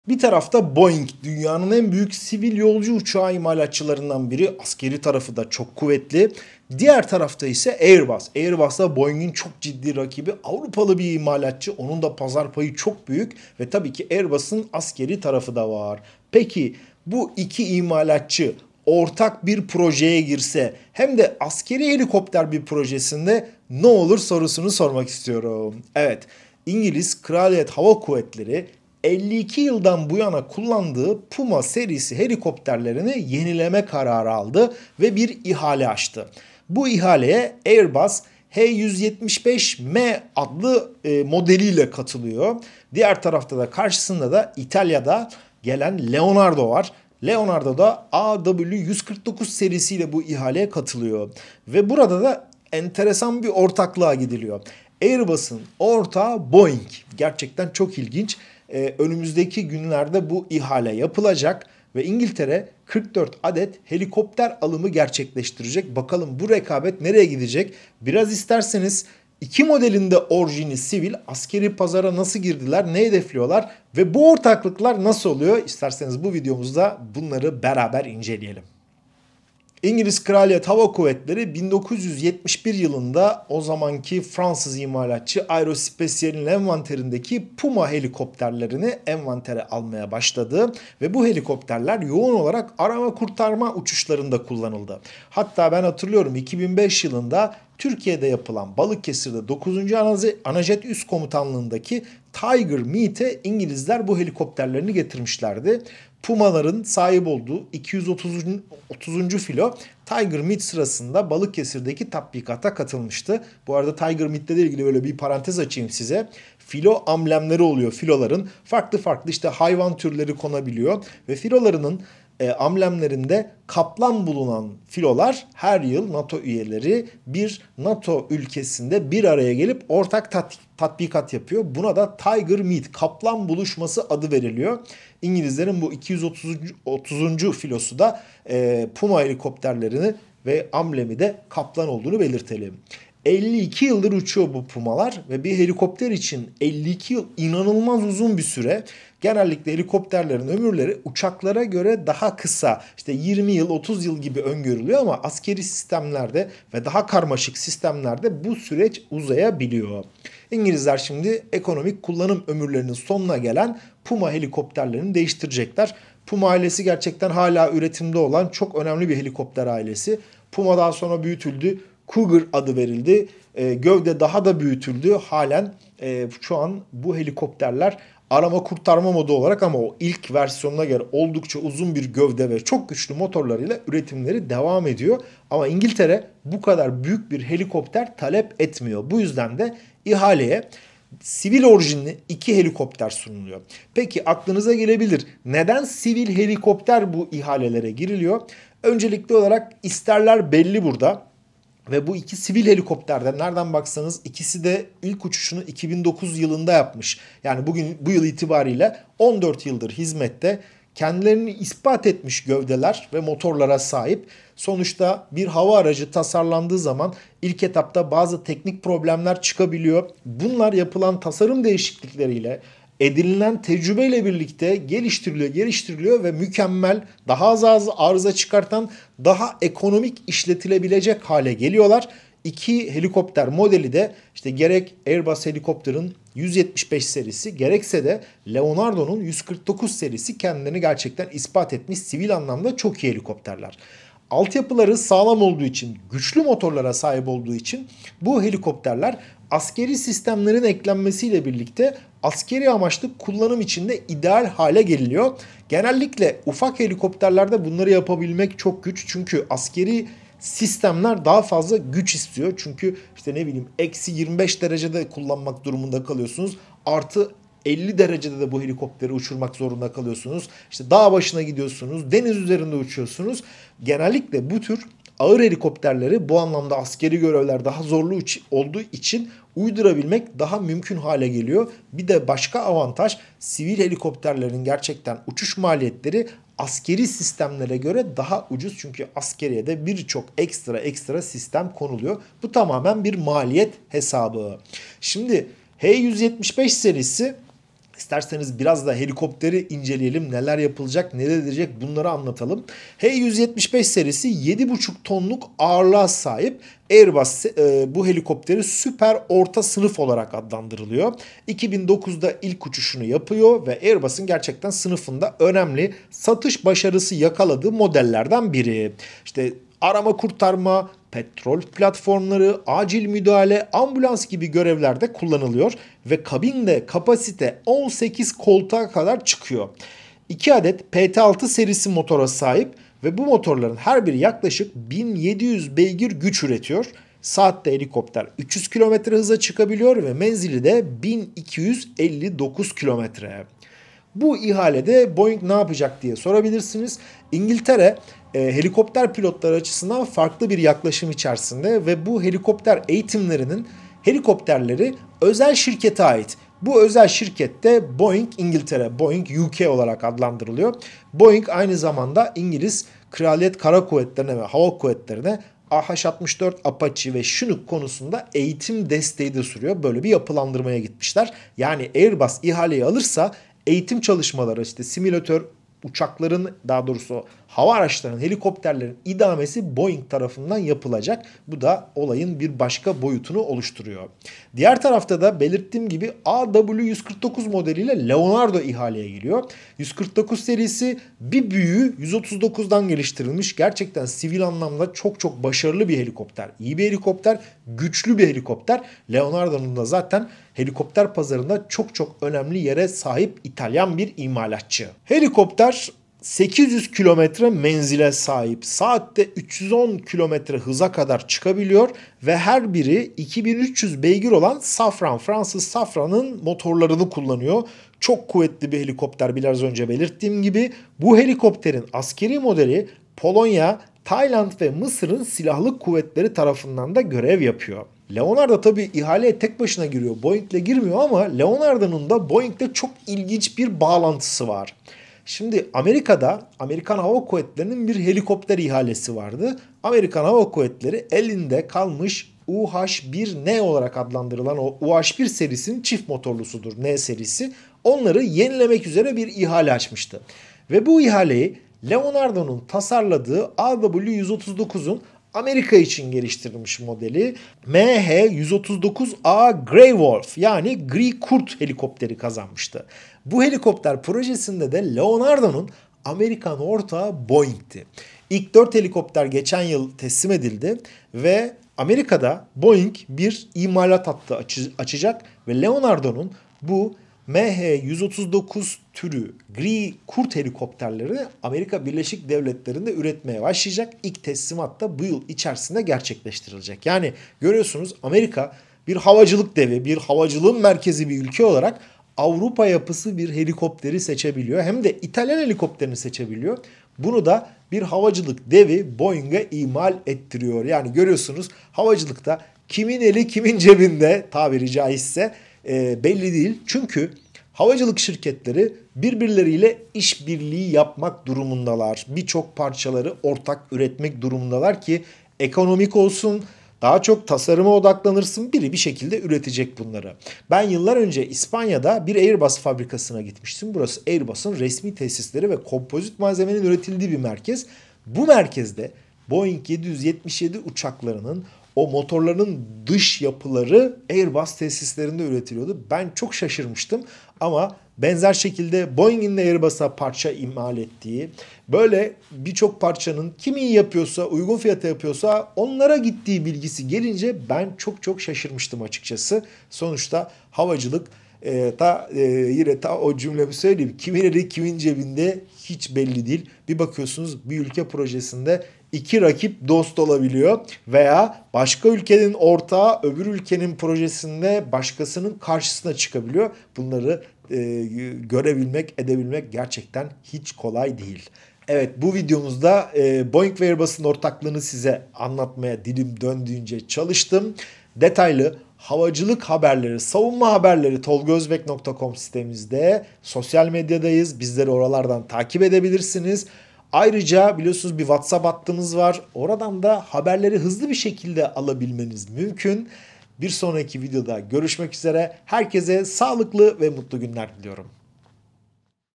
Boeing İngiltere’deki ihale için ezeli rakibi ile ortak oldu
Boeing ilginç bir adım attı. İngiltere'de Kraliyet Hava Kuvvetleri'nin ihalesi için ezeli rakibi Airbus ile ortak oldu. İngiltere Puma helikopterini yenilemek istiyor. Alternatifleri Airbus H175M ve İtalyan Leonardo ise AW149 ile sunuyor. Kanalımıza destek vermek için
Hafta içi her gün saat 19.00'da yeni video.
Videomuzun müziği, değerli takipçimiz Selçuk Çelebi tarafından hazırlandı:
Kanalı  @Ambient Music Selcuk Celebi   Eserlerine ulaşmak için: